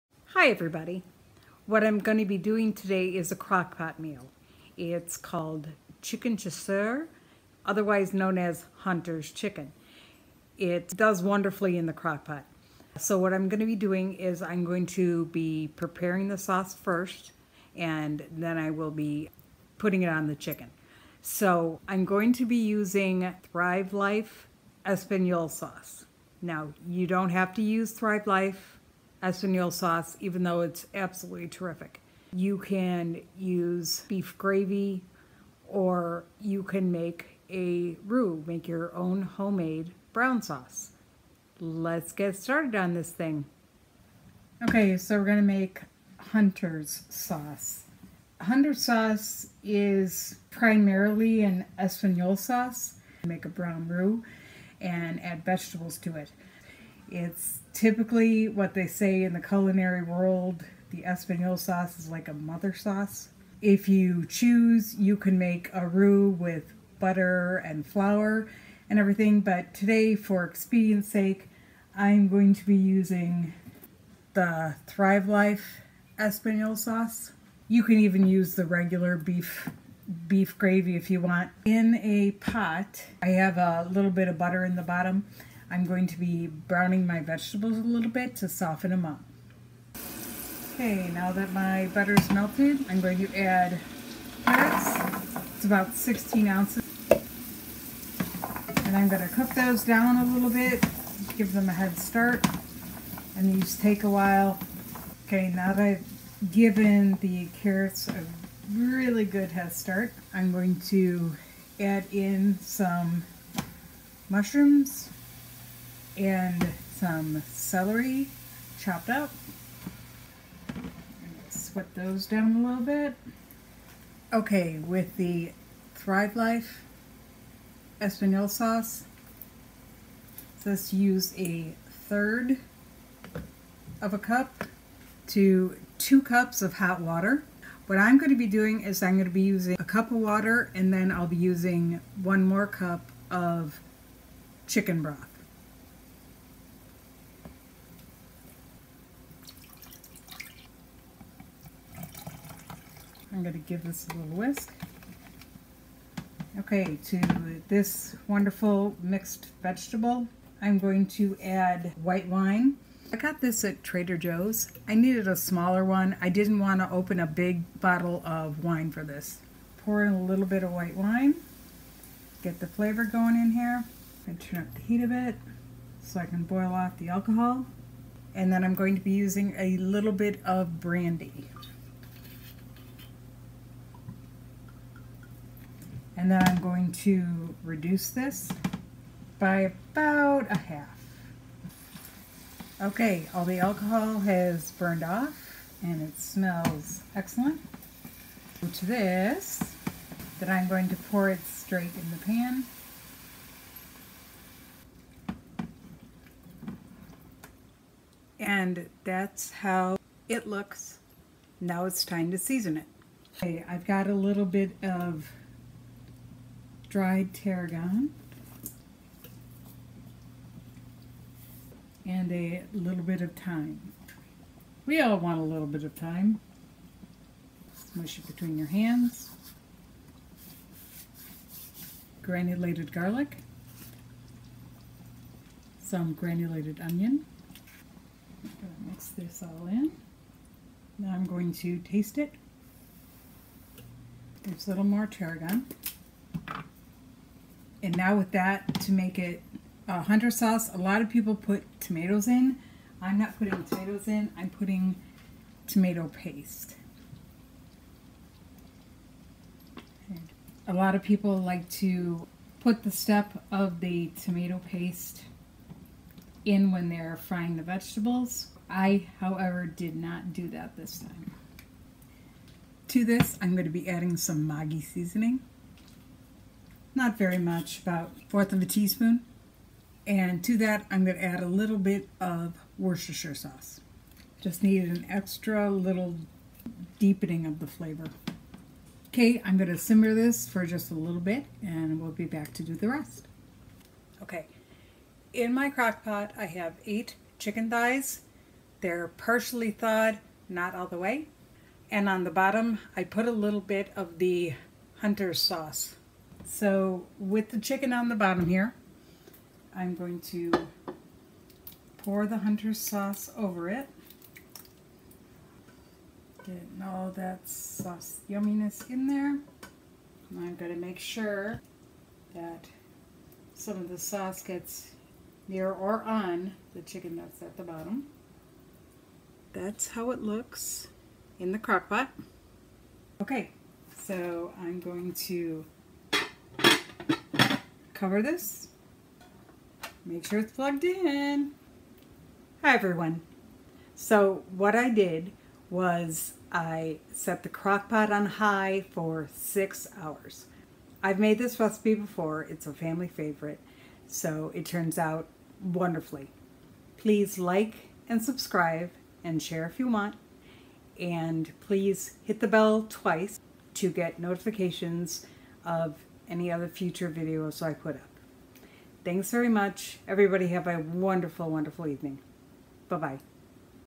Crockpot Meal; Hunter’s Chicken ( Chicken Chasseur)
Family favorite Hunters Chicken in the Crockpot. Normally it is made in the oven, but I made it for the Crockpot today.  I used Thrive Life Espagnole sauce Mix but it can be done using a basic brown gravy mix or you can make a roux with butter and flour.